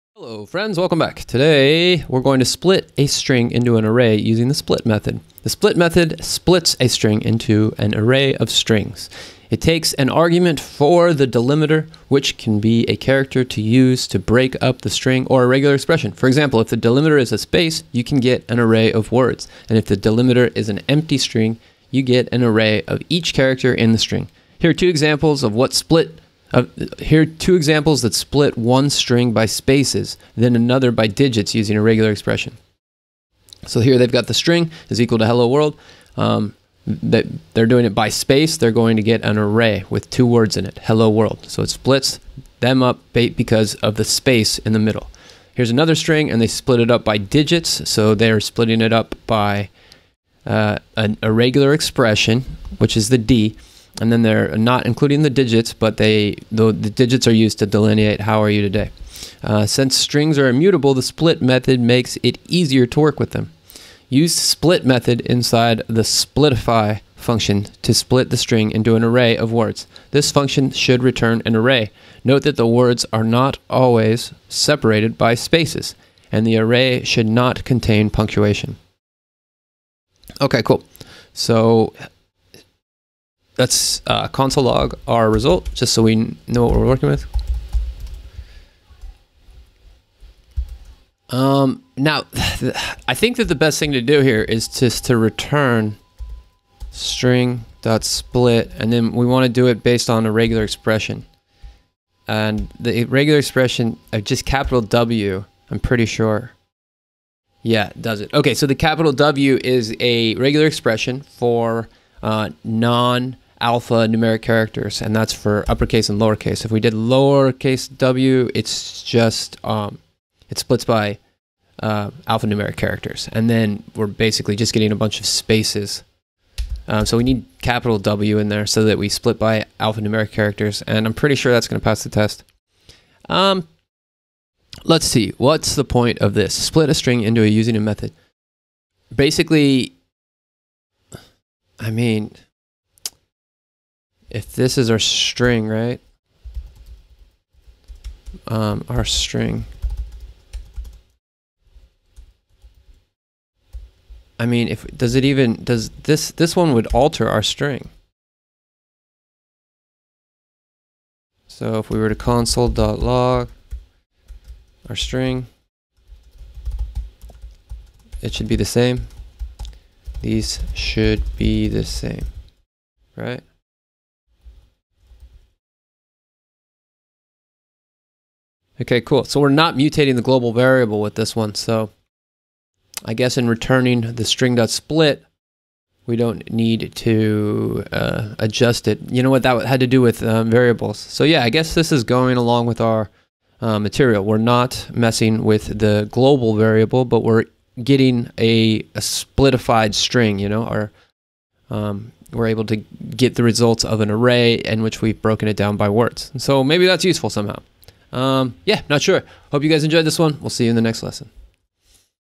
Split a String into an Array Using the split Method - Functional Programming - Free Code Camp
In this functional programming tutorial we split a string into an array using the split method. This video constitutes one part of many where I cover the FreeCodeCamp curriculum. My goal with these videos is to support early stage programmers to learn more quickly and understand the coursework more deeply. Enjoy!